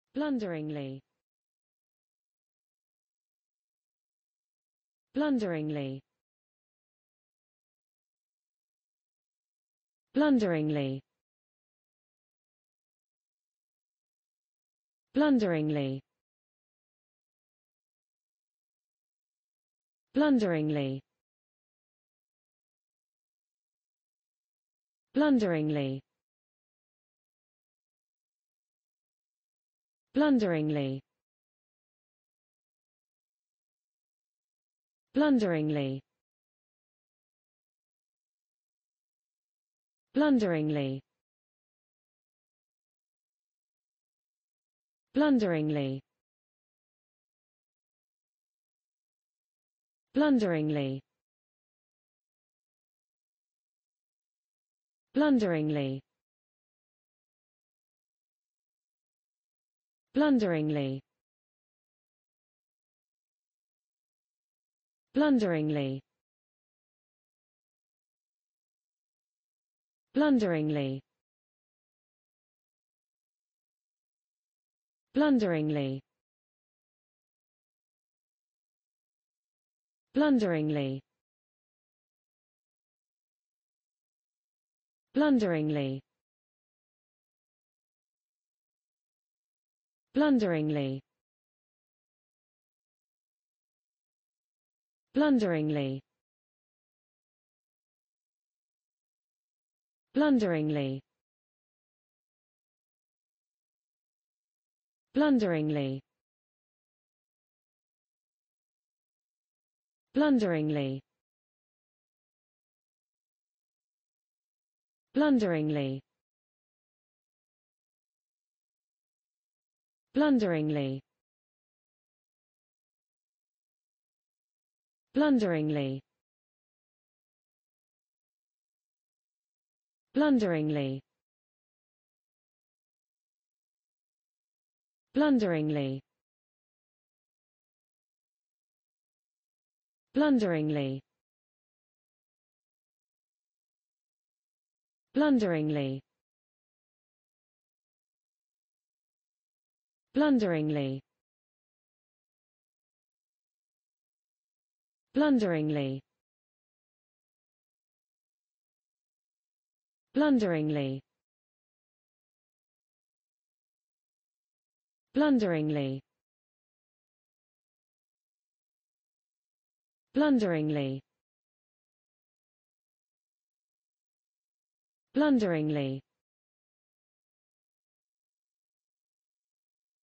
blunderingly. How to pronounce the english word blunderingly .Start with B. Learn from me.
Follow me!Learn from me how to pronounce the english word blunderingly.This video shows you how to pronounce the English word blunderingly .